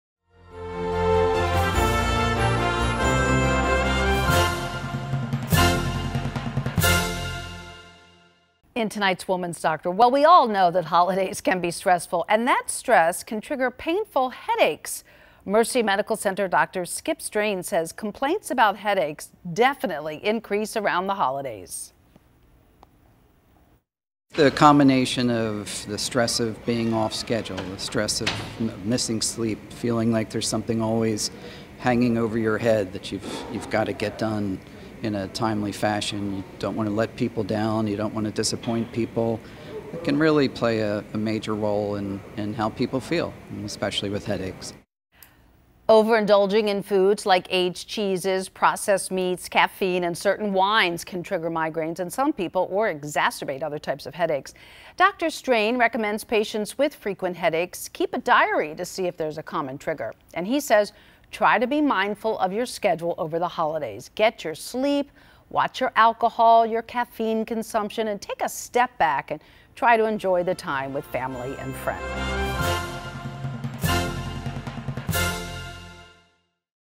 Headaches Increase Around the Holidays, Here's What To Do - Dr. Francis "Skip" Strain - Mercy
Dr. Skip Strain discusses why headaches can increase around the holidays and how to avoid them.